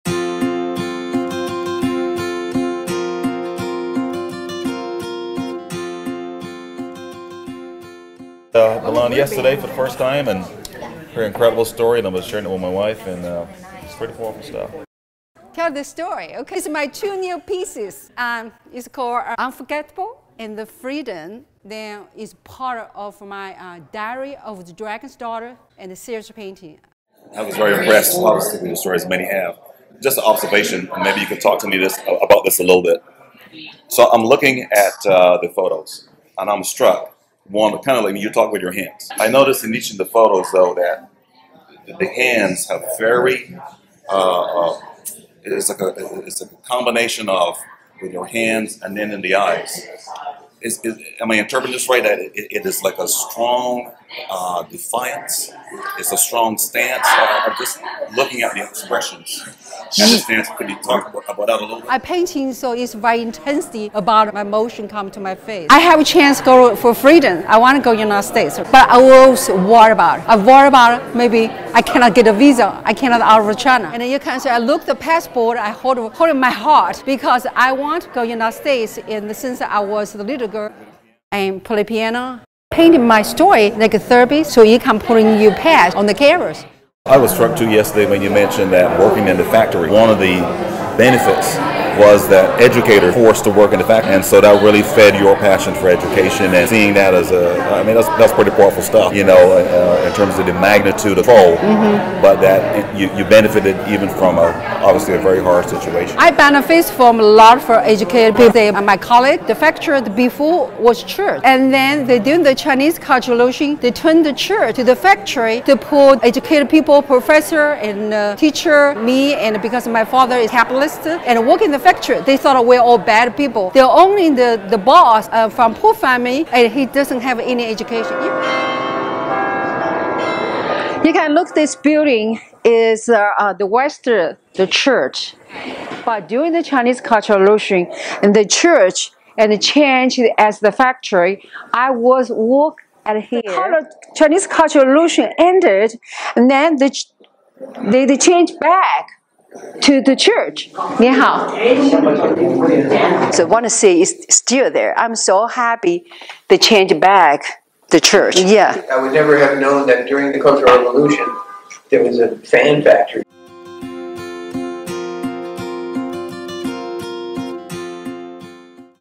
Conversation with WKCTC President Anton Reece
Conversation with WKCTC President Anton Reece during the art exhibition Paducah School of Art Design's gallery on October 13, 2017.